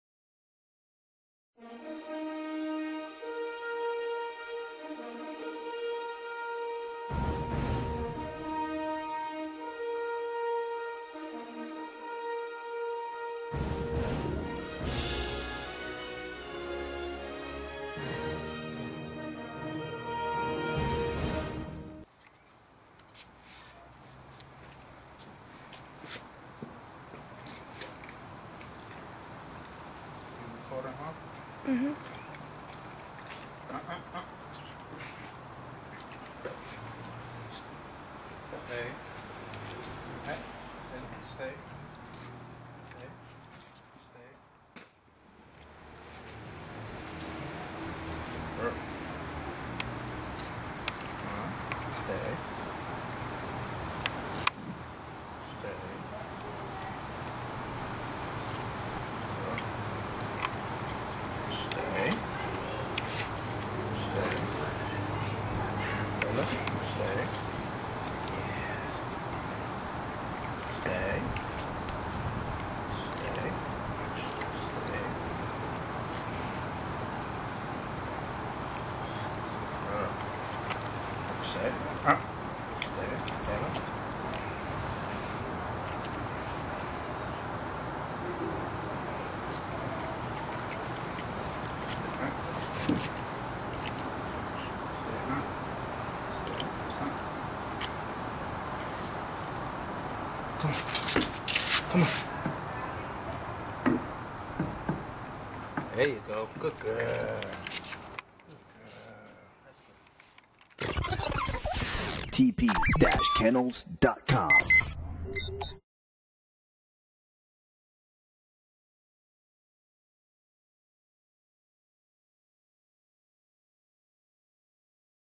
STACK TRAINING WITH CANS.
CH. LAYLA DID WELL FOR HER FIRST TIME stacking on cans.